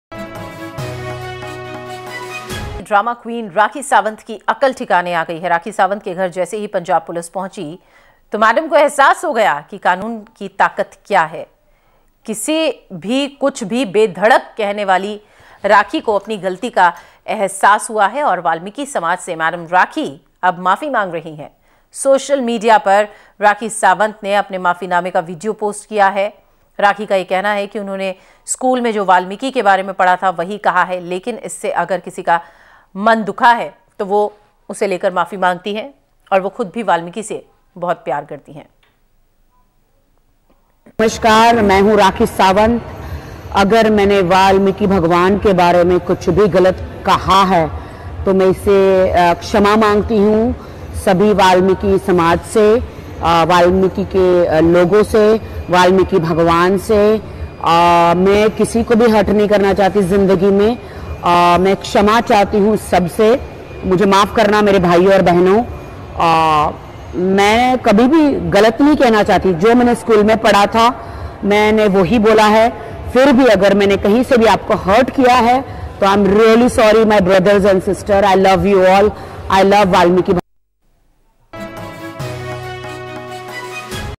Viral video: Rakhi Sawant apologise to Balmiki society
Video viral of Film & TV actress and Item girl Rakhi Sawant apologized to Balmiki society...